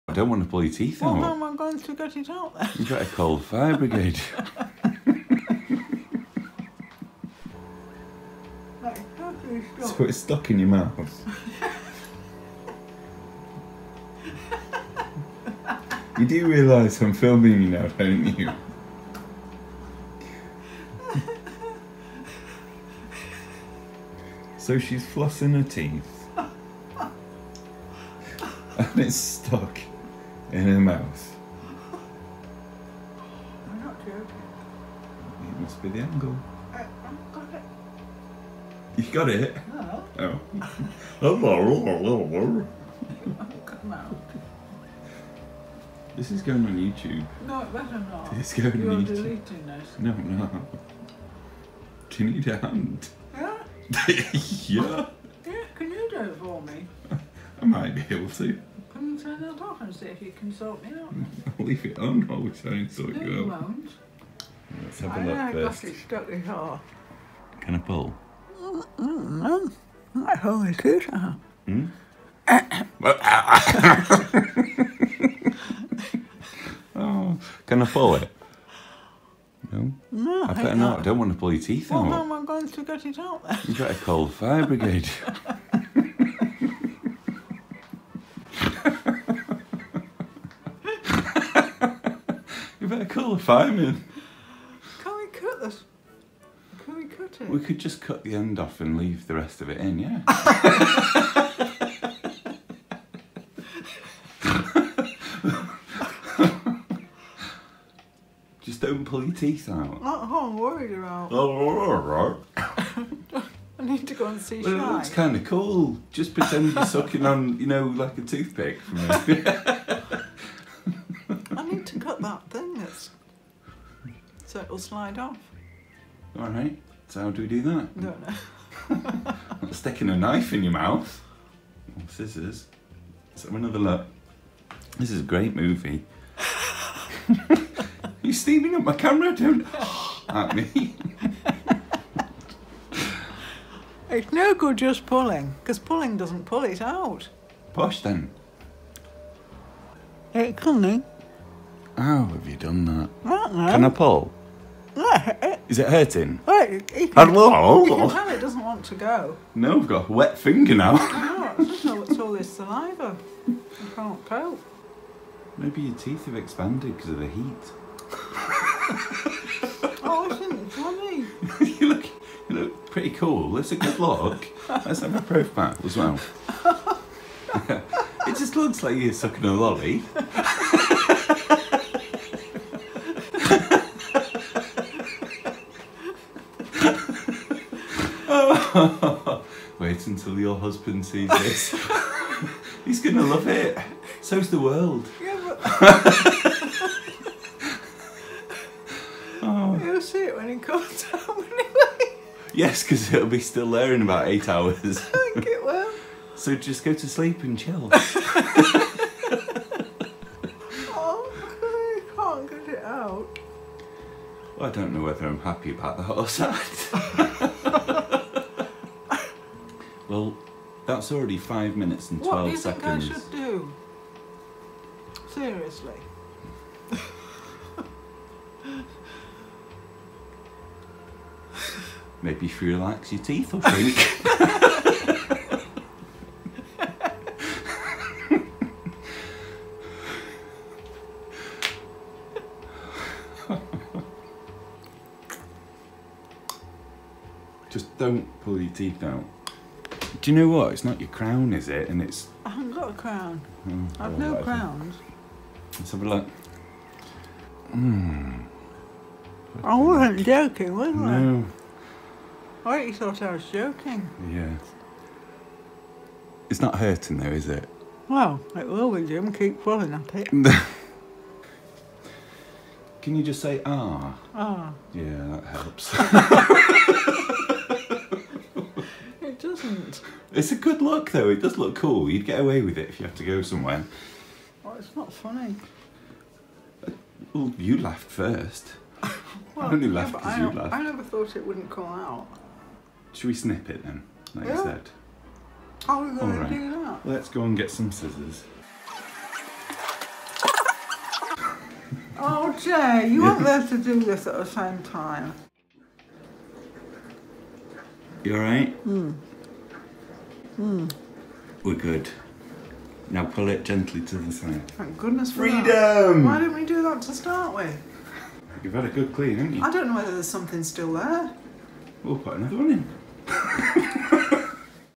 Dental Flosser Stuck In Mum's Mouth (Funny Video) 😂😂
Mum has a dental flosser stuck in her mouth. Hard not to laugh throughout this drama!

If you liked the David After Dentist viral video, you'll love this one too!

Hilarious Video
YouTube Video
Funny Mum
Hilarious Video
Comedy